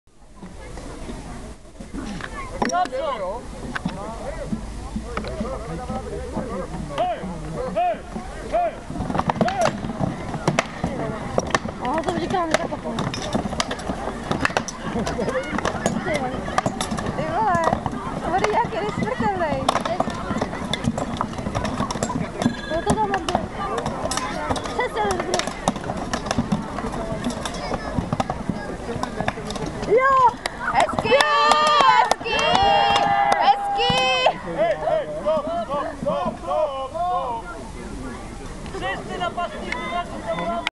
Iłża 2008 Zamczysko 1
Pierwsze starcie... i ostatnie w którym zakonni zostali pokonani!